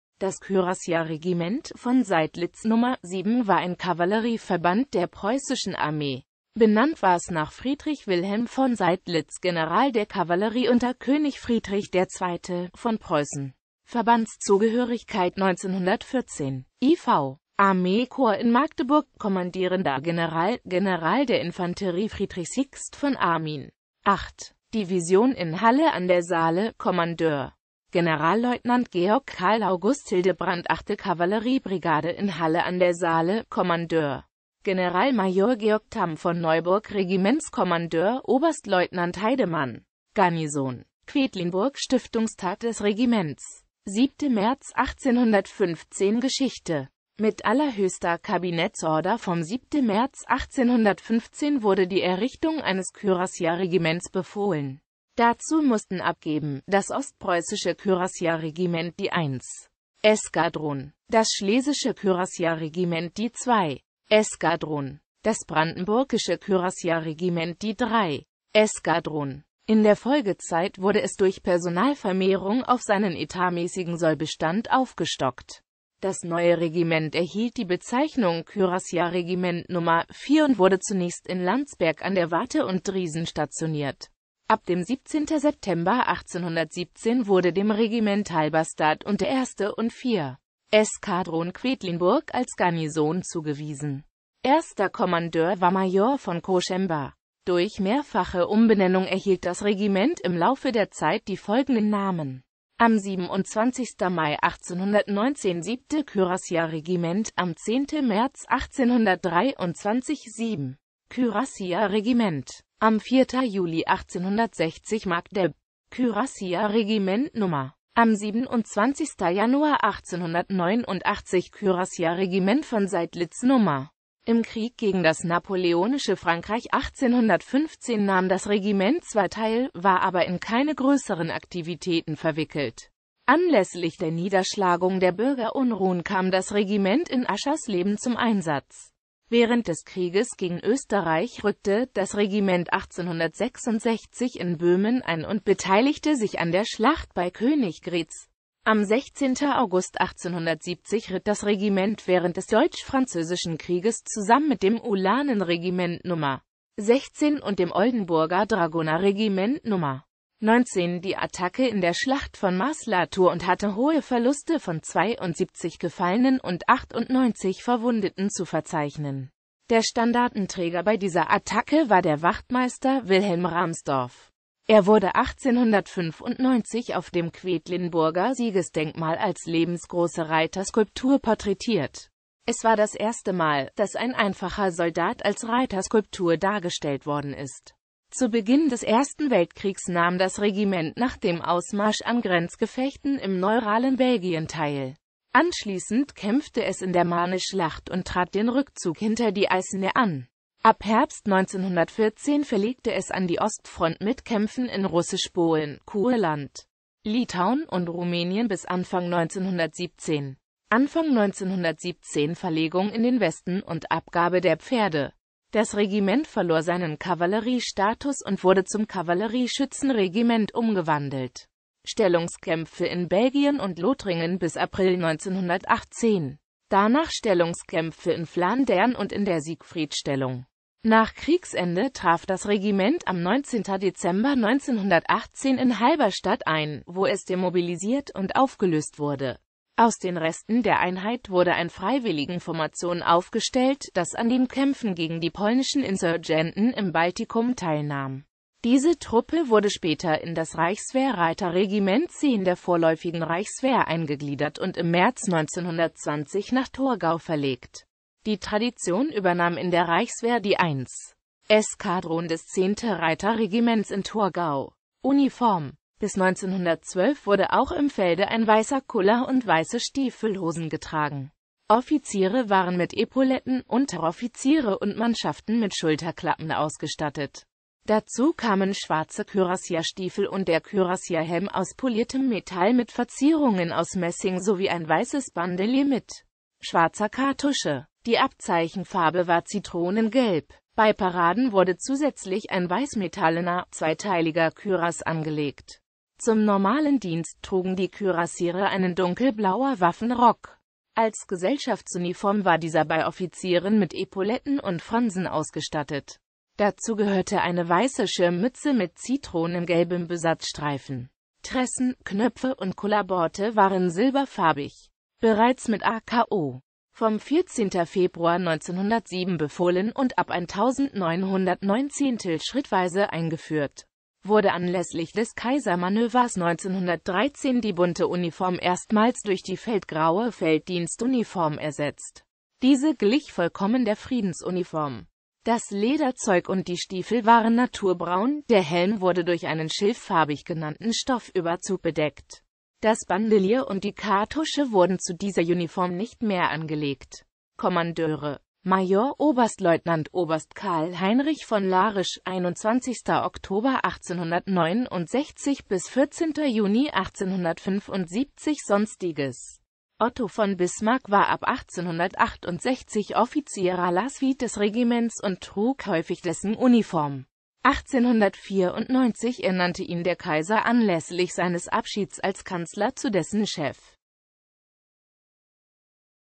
Kürassier-Regiment „von Seydlitz“ (Magdeburgisches) Nr. 7
Vielen Dank für Ihre Unterstützung:

Das Kürassier-Regiment „von Seydlitz“ Nr.7 war ein Kavallerieverband der Preußischen Armee.Benannt war es nach Friedrich Wilhelm von Seydlitz, General der Kavallerie unter König Friedrich II.von Preußen.